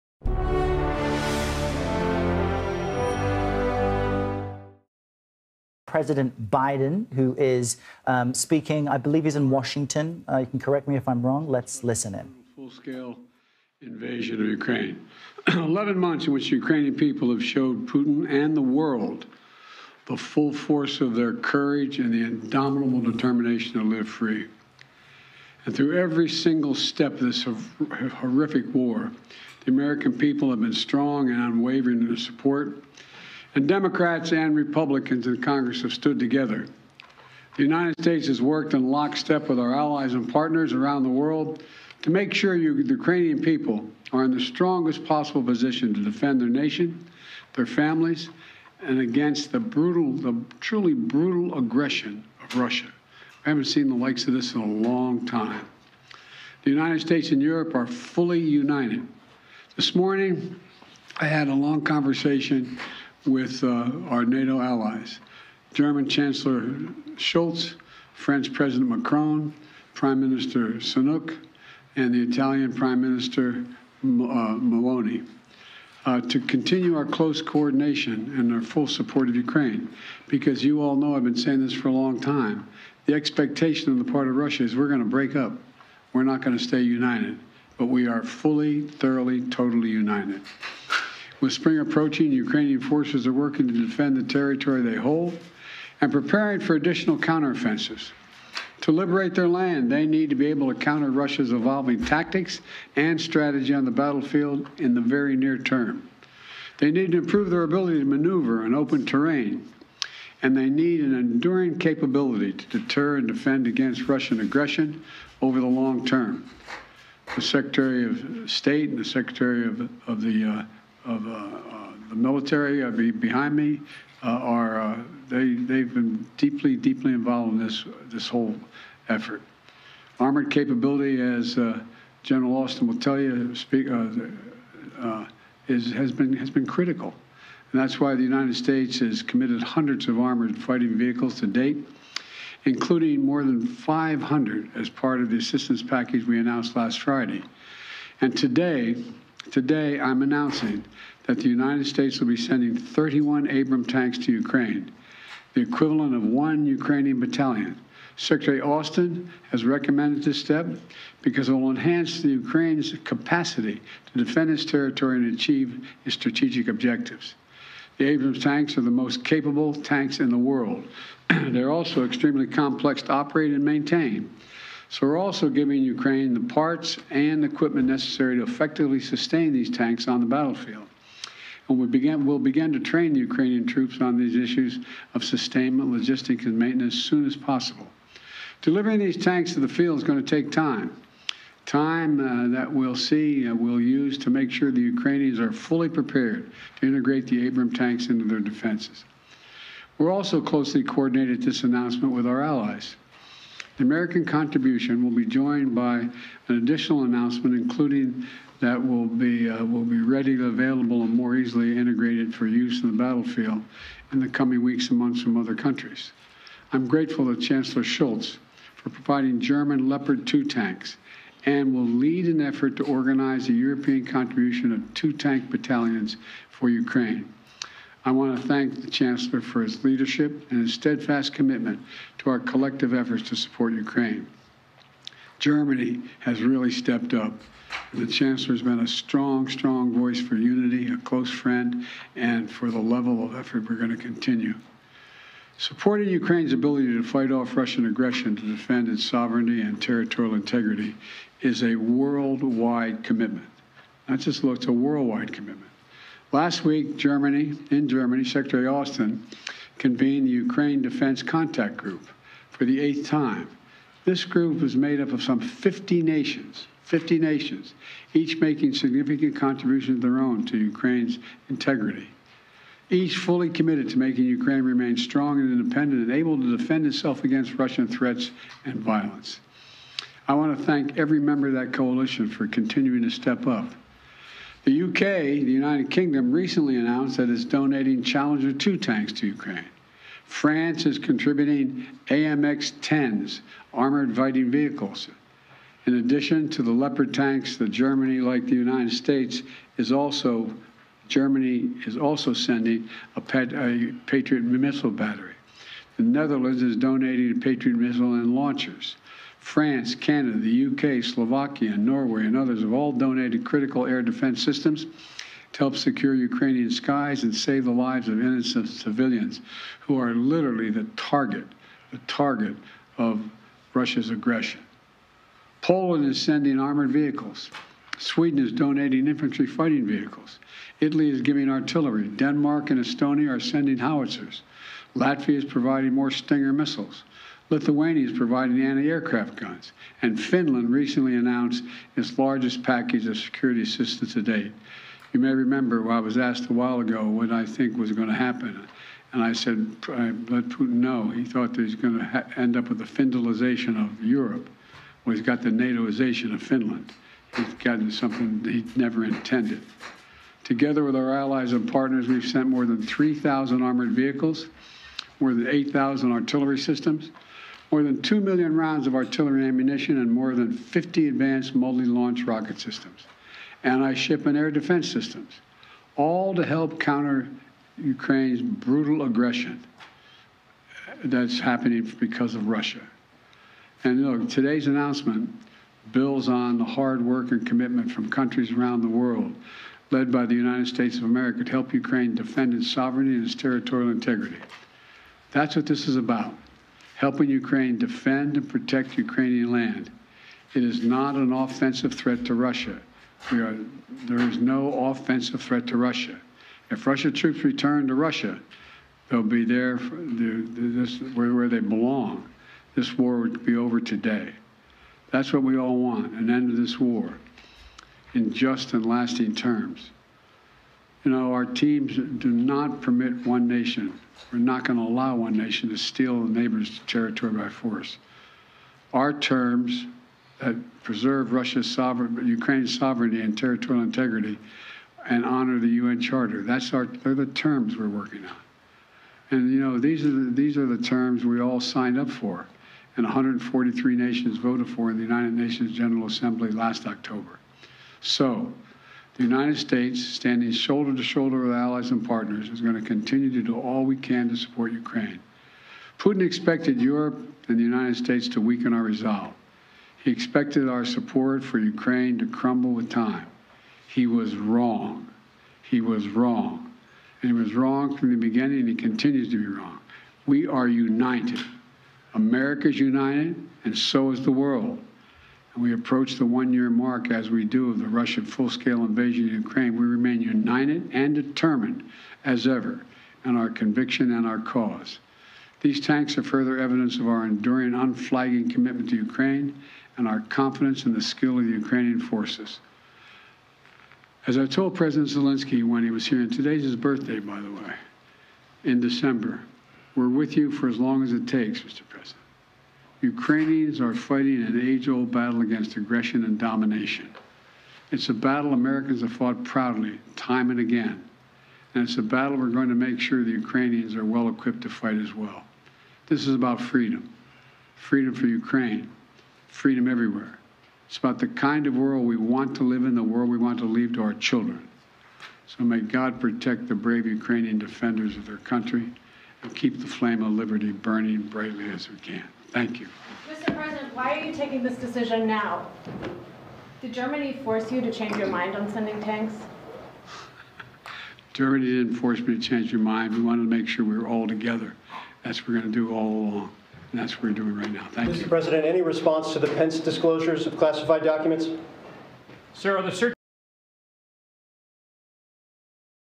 news vlogging BIDEN TO SEND 31 ABRAMS TANKS TO UKRAINE
Breaking Political News BIDEN TO SEND 31 ABRAMS TANKS TO UKRAINE